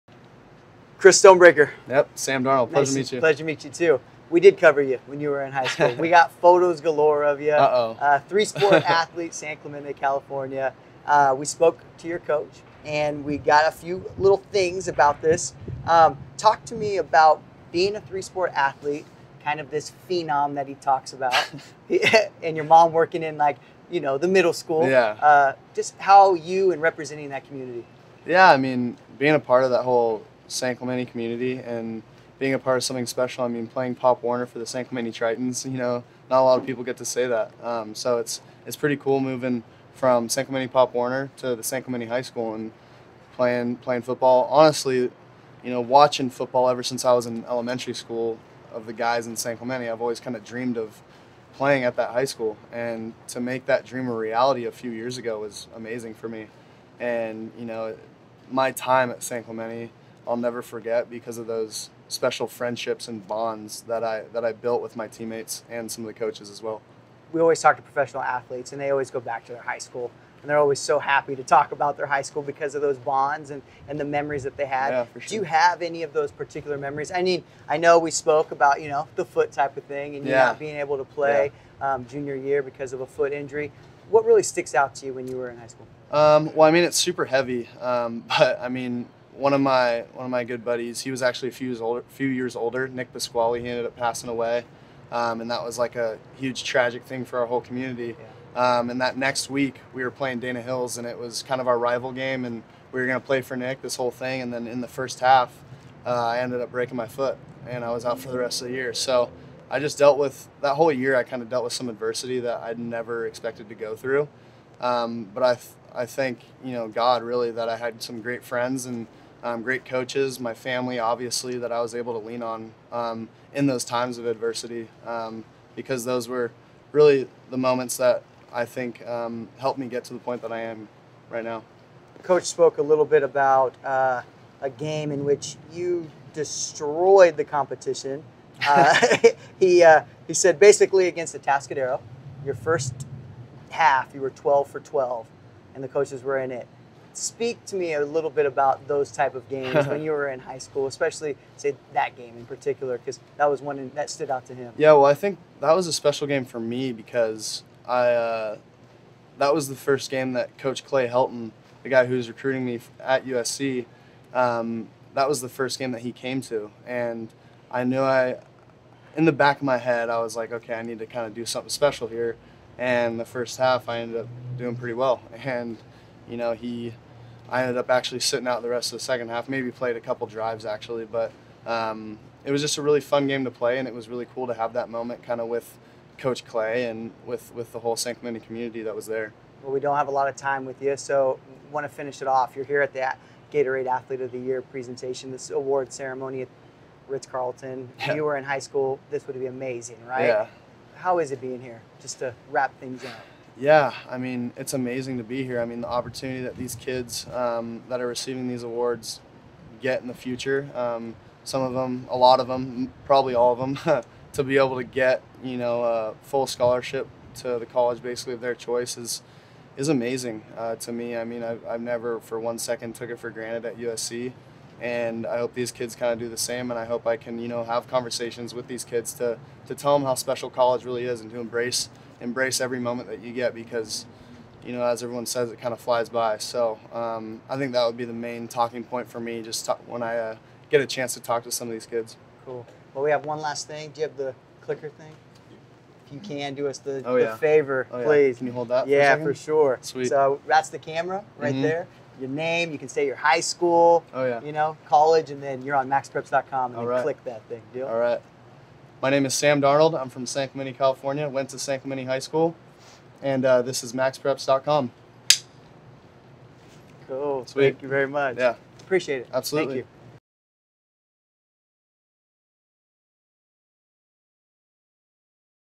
Sam Darnold Interview
NY Jets starting quarterback Sam Darnold sits down with Chris Stonebraker of MaxPreps at the Gatorade Athlete of the Year awards ceremony.